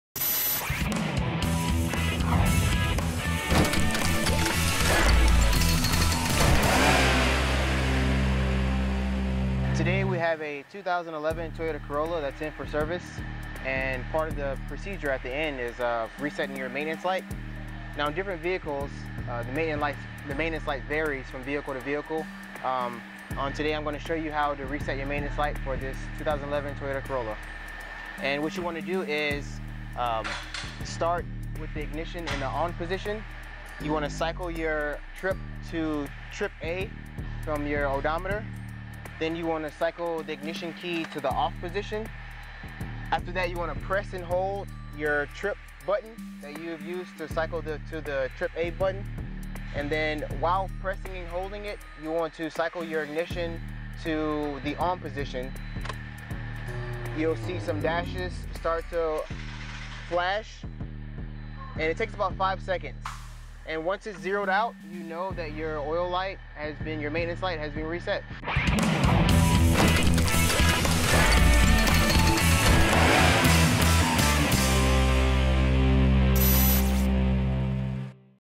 Resetting Your Maintenance Light
Master Technician DeJuan Sutton explains how to reset your Toyota maintenance light.